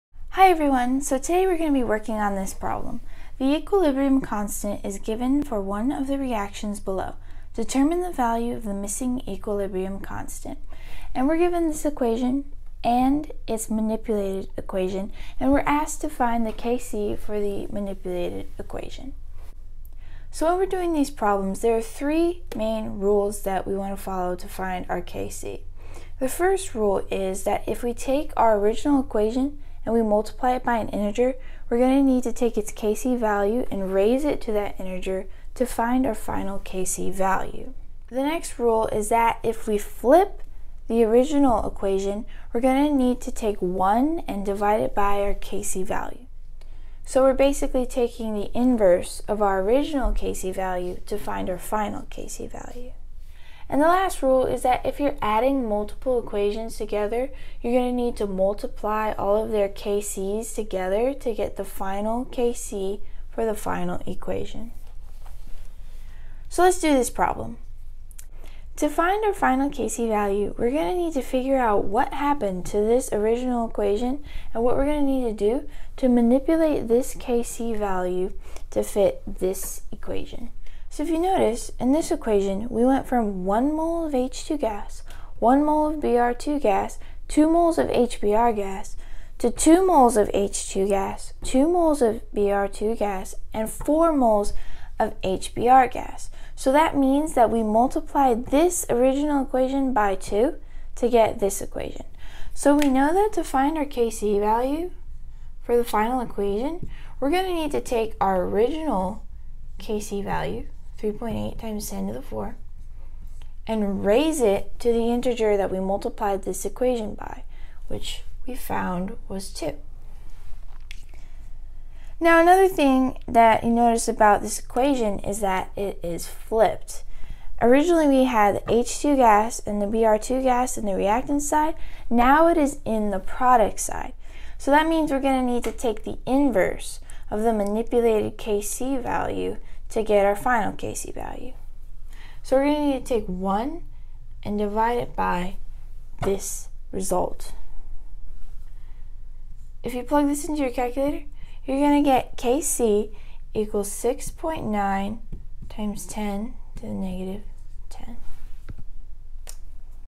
Determining the Value of the Missing Equilibrium Constant
This video explains the rules to find missing equilibrium constants and how to use them in different reactions.